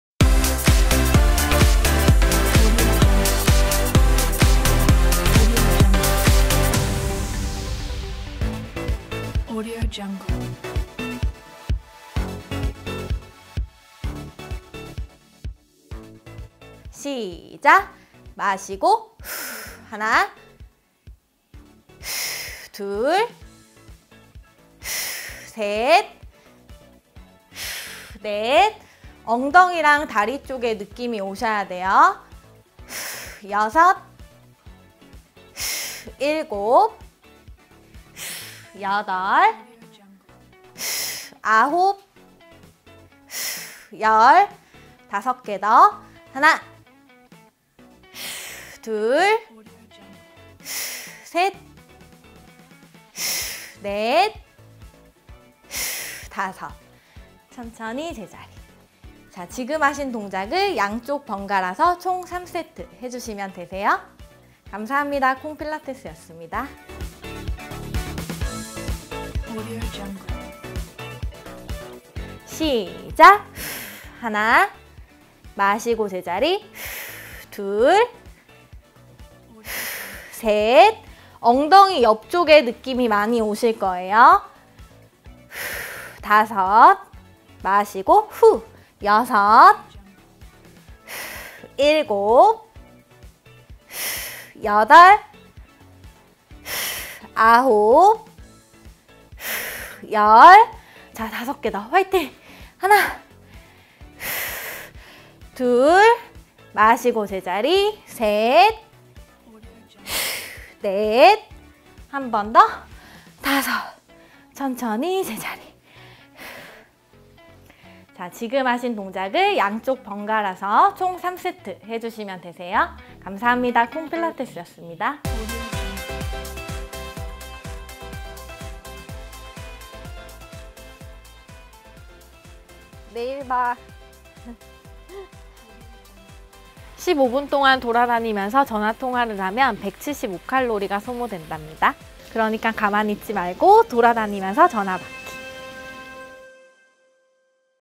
6일차, 하체비만탈출! 1주차 마무리 | 2017 영바디키 운동 | 뉴트리라이트
이번주 하체비만 탈출운동 한번 더 따라해볼까요?